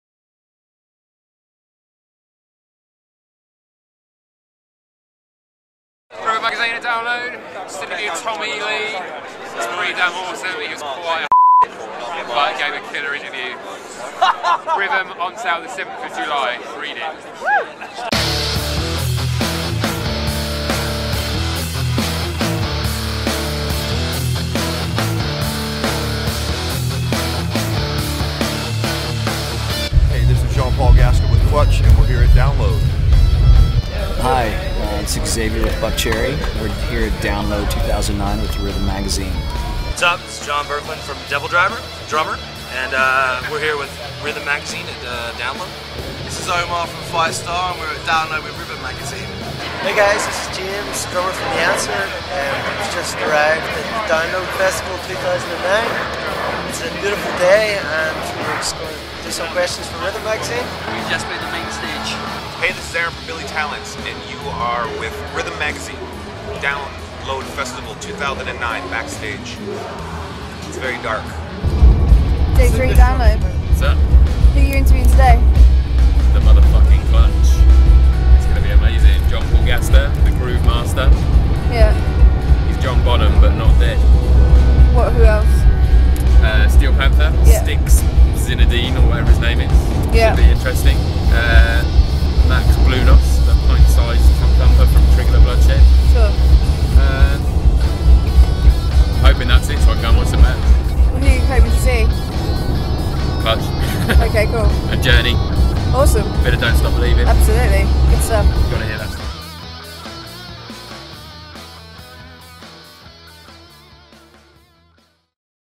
Rhythm magazine's Download 2009 highlights
Backstage at Download 2009 with Rhythm magazine and a host of the best drummers around. For more from the festival, check out Rhythm's Tommy Lee/Wildest Drummers Ever issue, onsale 7 July. For more video from Download 2009, check out